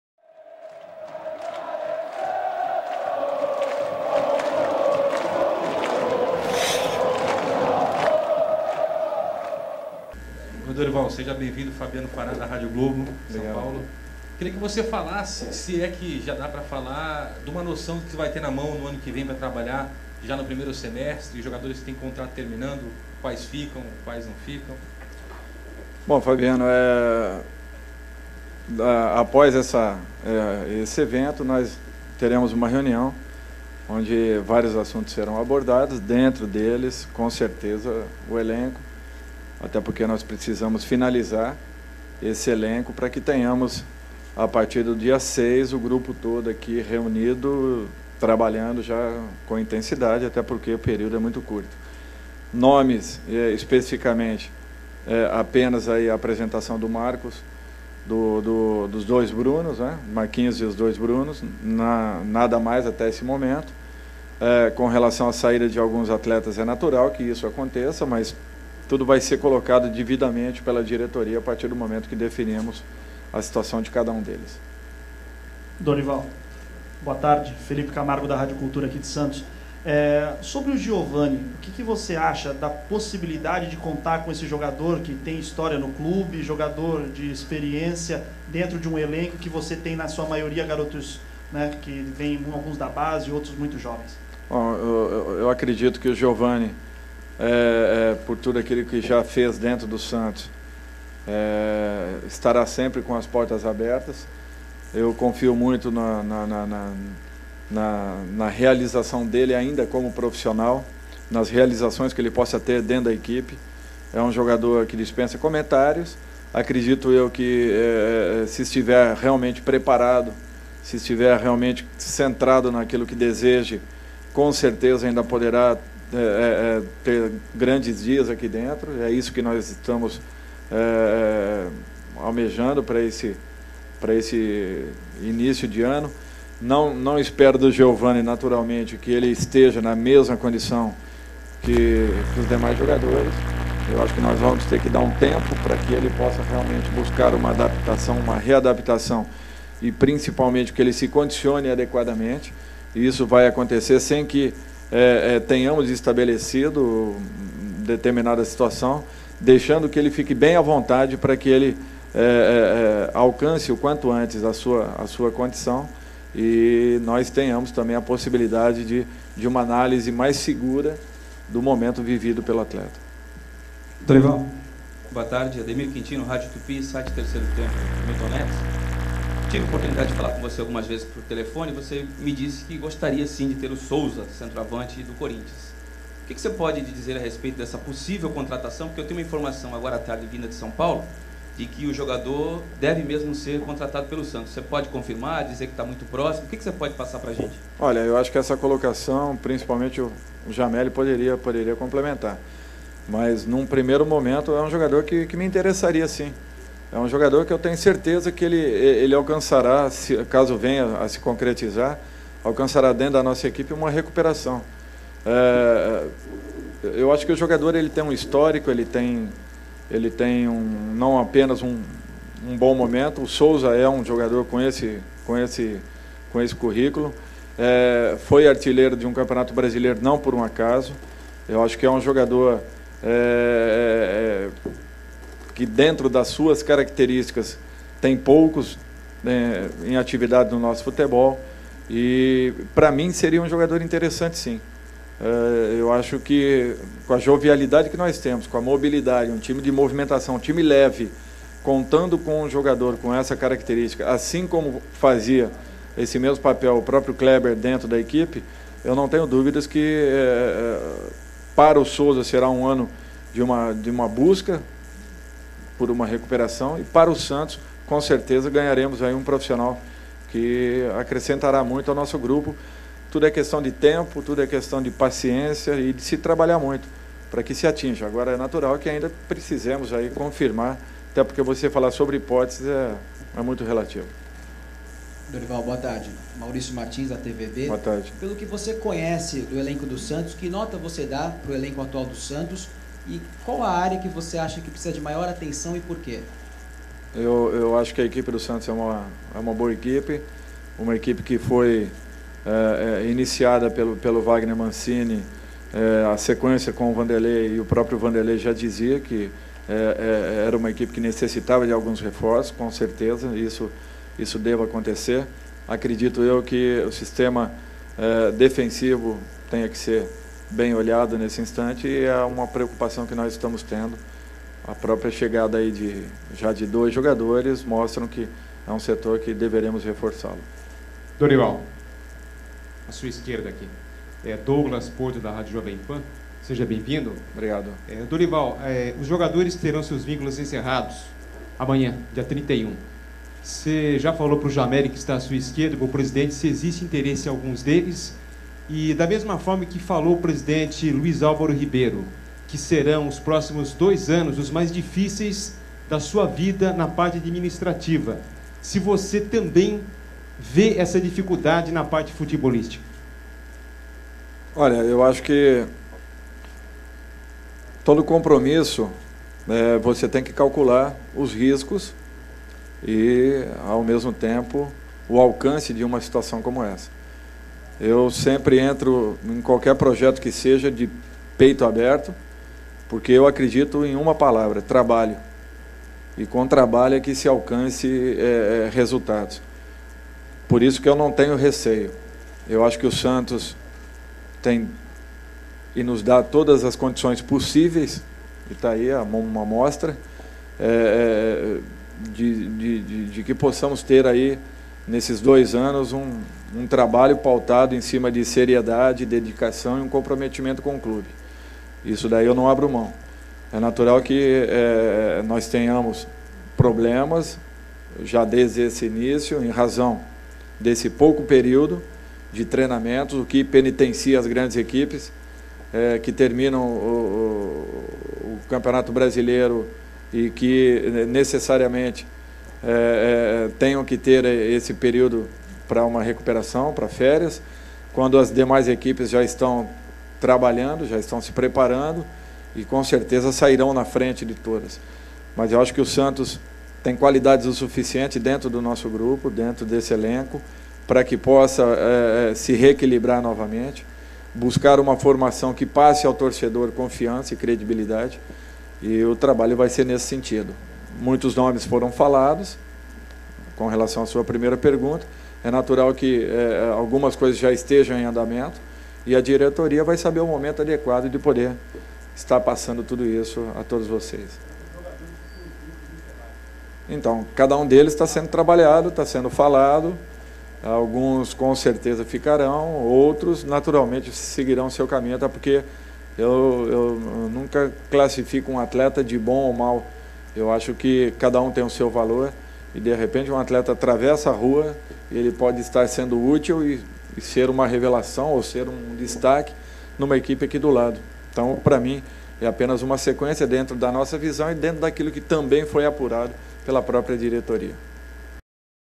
Apresentação do Técnico Dorival Jr. (parte 1)
Confira a íntegra da entrevista coletiva do novo técnico do Santos Futebol Clube, Dorival Júnior. A apresentação aconteceu no dia 30/12/2009.